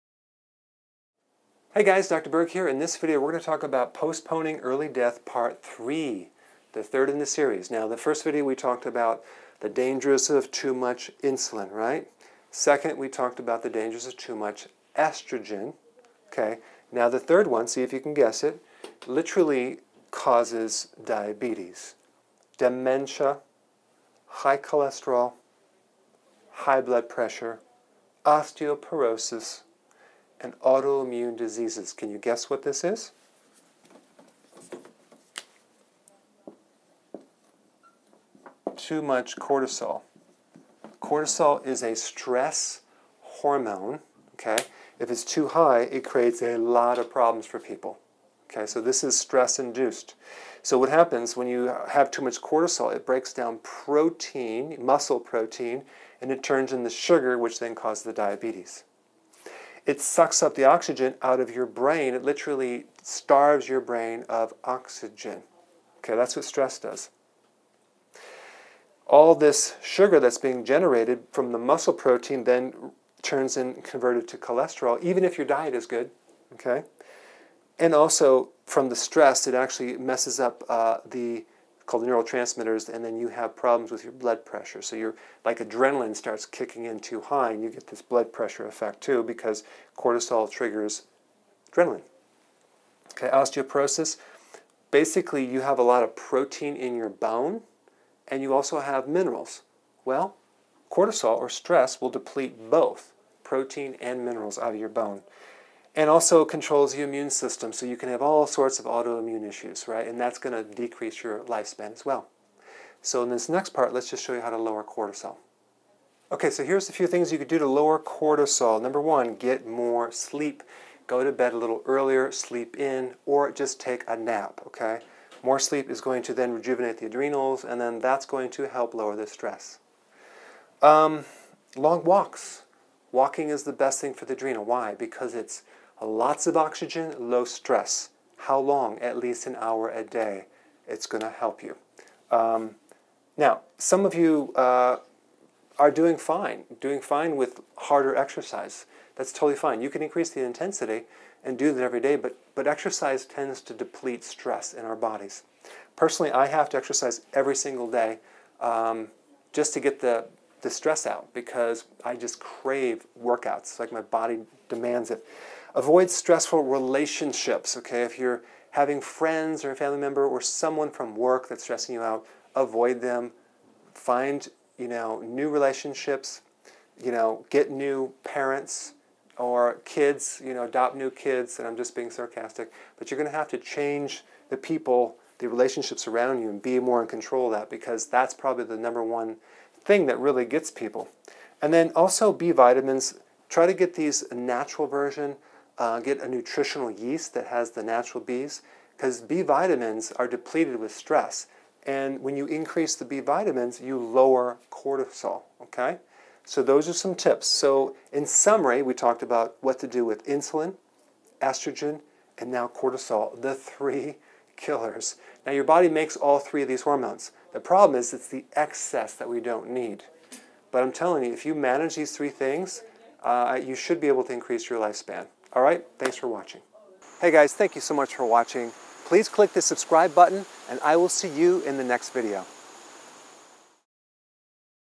How To Prevent Premature Death & Increase Longevity – Part 3 By Dr.Berg
Here are some tips to help postpone premature death and help increase longevity. Enjoy part 3 of this 3 part series.

Timestamps
0:15 The dangers of high cortisol
0:35 What is cortisol?
1:55 How to lower cortisol

In this video, we’re going to talk about postponing premature death and potentially increasing longevity. This is part 3 in an important 3 part series. In part 1 we talked about the dangers of too much insulin. In part 2 we talked about the dangers of too much estrogen. Now we’re going to cover cortisol.

Cortisol is a stress hormone. If it’s too high, it can create a lot of problems, including:

 • Diabetes
 • Dementia
 • High cholesterol
 • High blood pressure
 • Osteoporosis
 • Autoimmune diseases

How to help lower cortisol:

1. Get more sleep
2. Take long walks
3. Avoid stressful relationships
4. Get plenty of B-vitamins (nutritional yeast)

Consider following these tips to help postpone premature death and increase longevity.